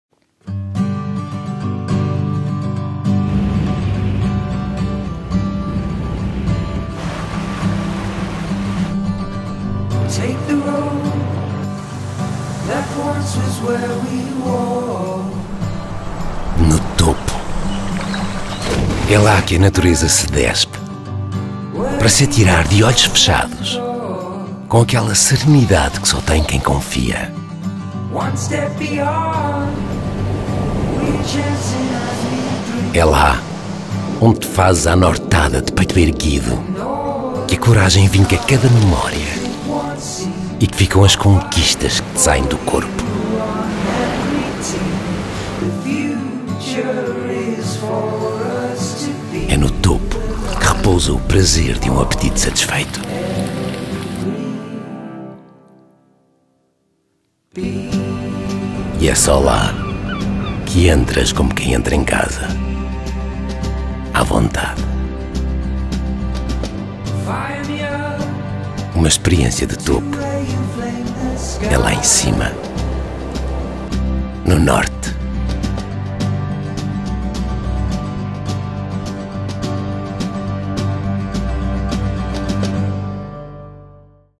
O Norte está lá em cima!
O filme que promove o turismo da região do Porto e Norte conta uma história de diferentes viagens lá acima. Ao Norte.

Por terra, uma família ruma ao topo do país, entrando pelas Montanhas Mágicas, fazendo a ponte entre o Centro e o Norte de Portugal. Mas há diferentes formas para chegar ao Norte. Histórias com aventura, água, história, sabor, onde a natureza cobre de verde a paisagem. São percursos em busca de experiências de topo, lá em cima. No Norte.
The film which promotes tourism in the Porto and North region tells a story of different trips up there. To the North.

Travelling by land, a family heads to the top of the country, entering through the Magic Mountains, creating a bridge between the Centre and the North of Portugal. But there are different ways to get to the North. Stories with adventure, water, history, flavour, where nature covers the landscape with green. They are routes in search of top experiences, up there. In the North.